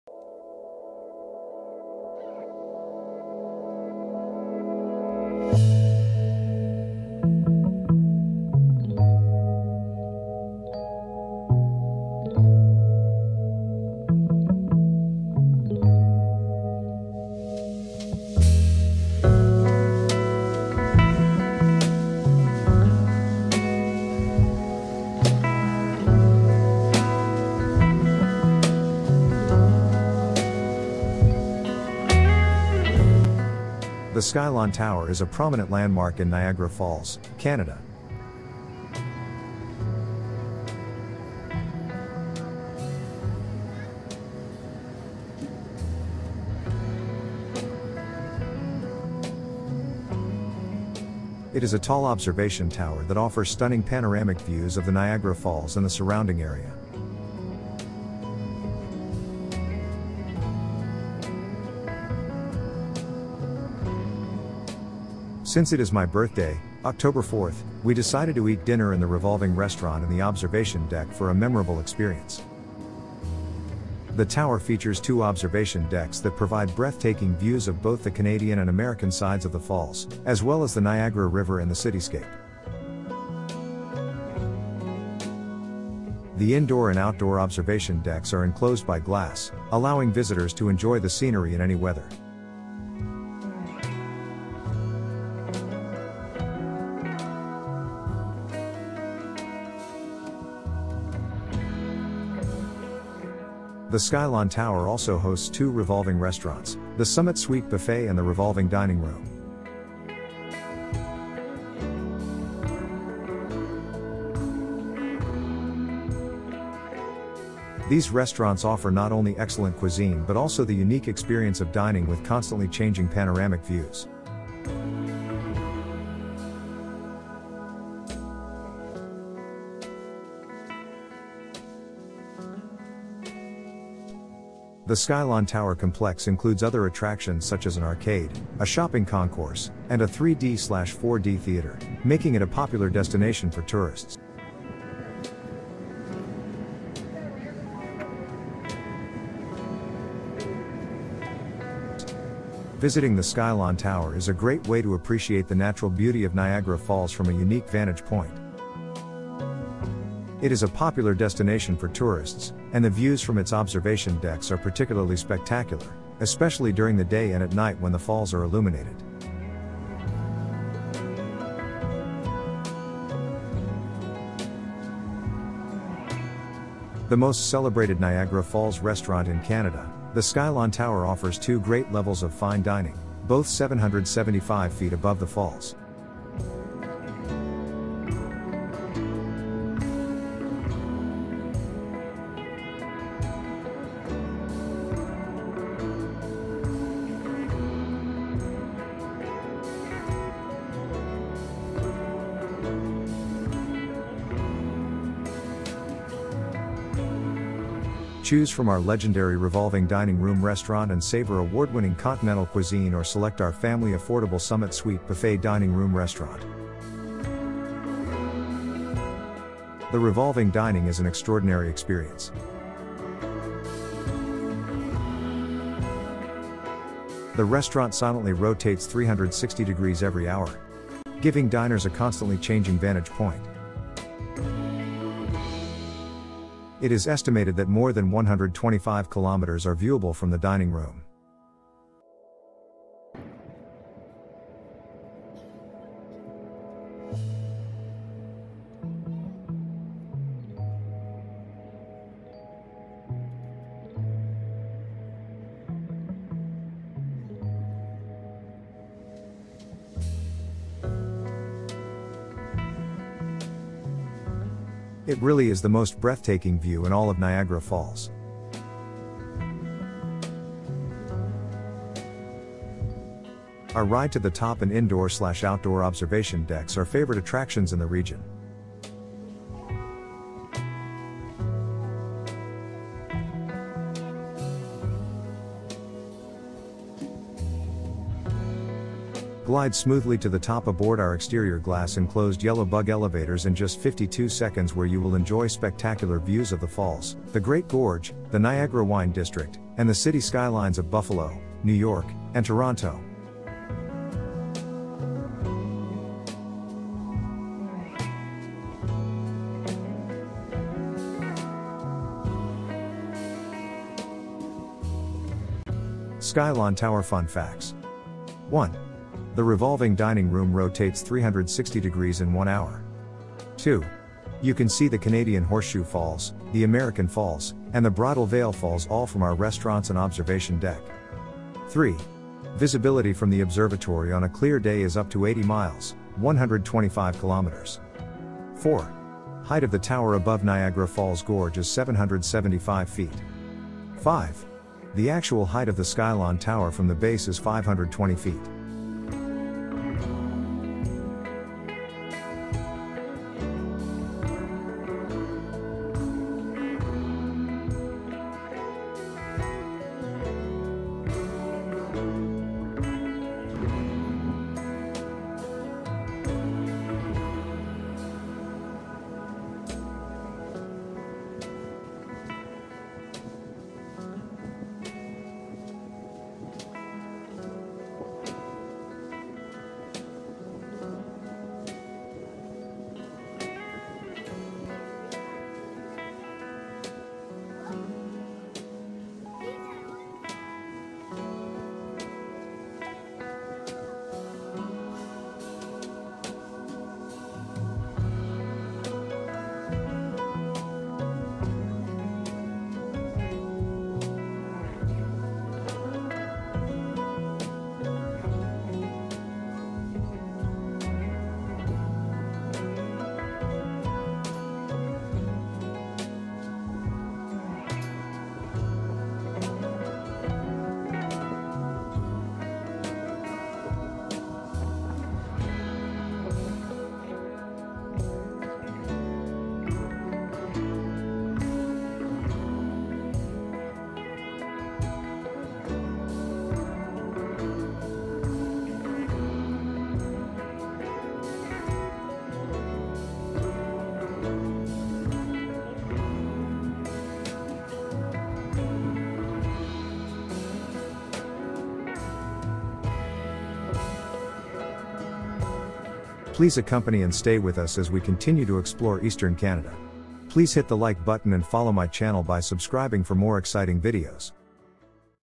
DINNER AT SKYLON TOWER 10-4-23
The Skylon Tower is a prominent landmark in Niagara Falls, Canada. It is a tall observation tower that offers stunning panoramic views of the Niagara Falls and the surrounding area.

Since it is my birthday, October 4th, we decided to eat dinner in the revolving restaurant in the observation deck for a memorable experience.